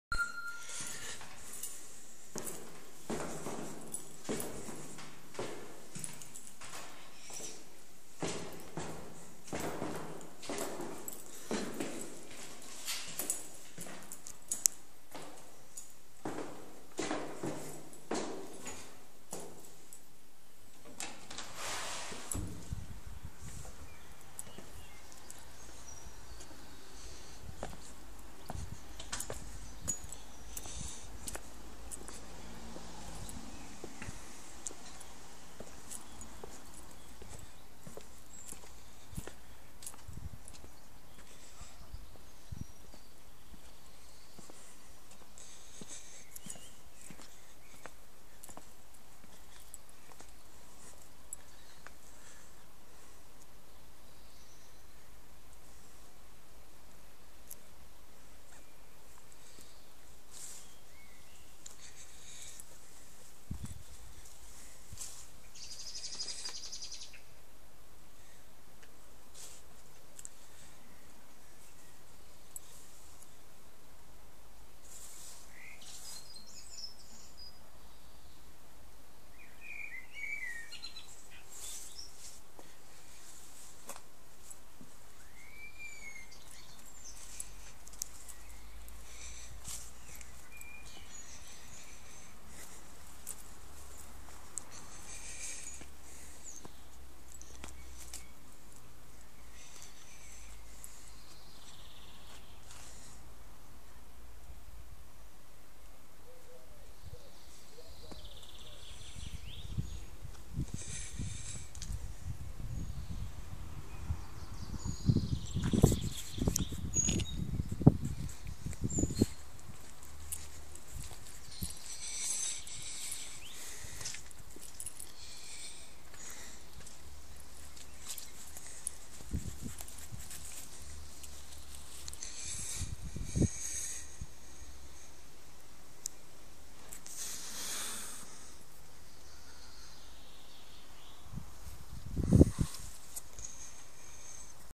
unsere Grünfläche
Nun einen wirklichen Sinn hat dieses Video nicht.
Aber ich dachte mir, ich zeige euch mal wie es so draußen ausschaut.